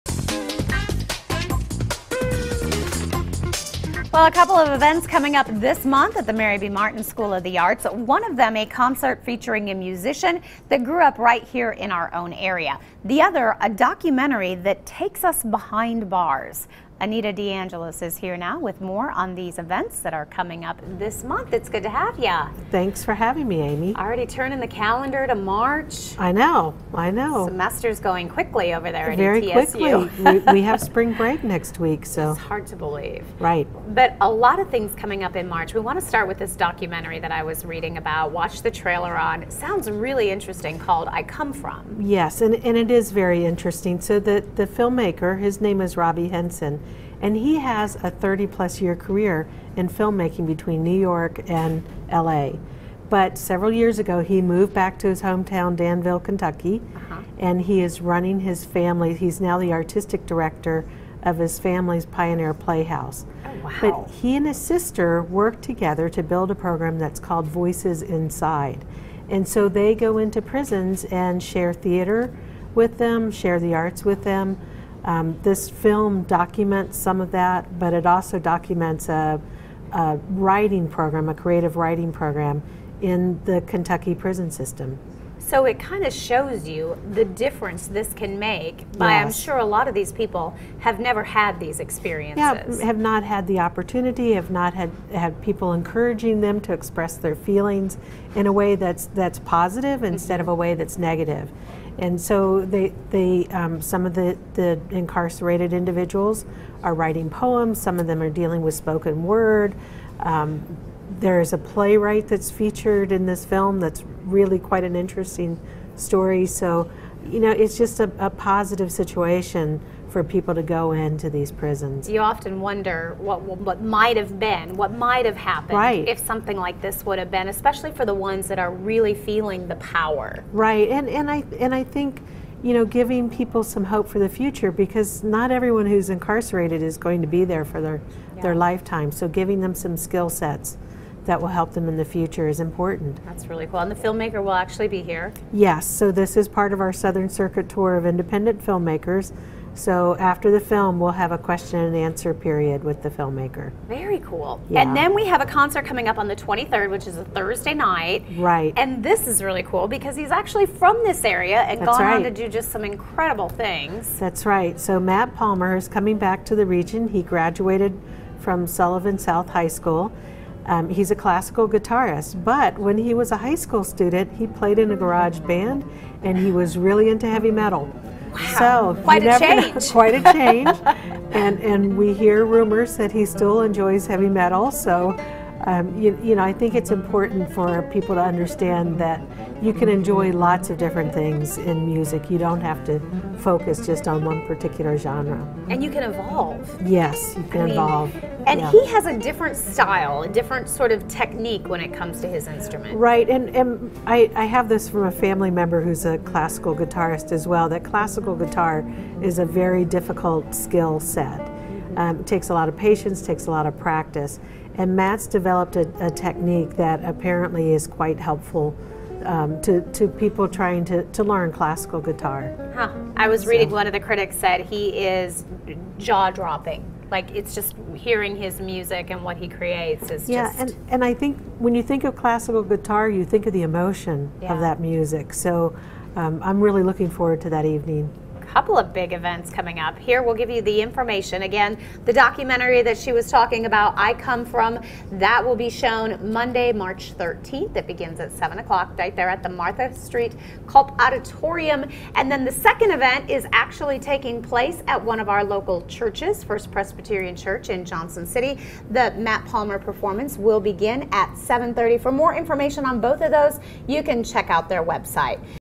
I Come From Documentary And Matt Palmer Perormance At Mary B. Martin
A couple of events are coming up this month at the Mary B. Martin School Of The Arts. One of which, is a concert featuring a musician that grew up right here in our area! The other is a documentary that takes us behind bars. Anita DeAngelis is here with more.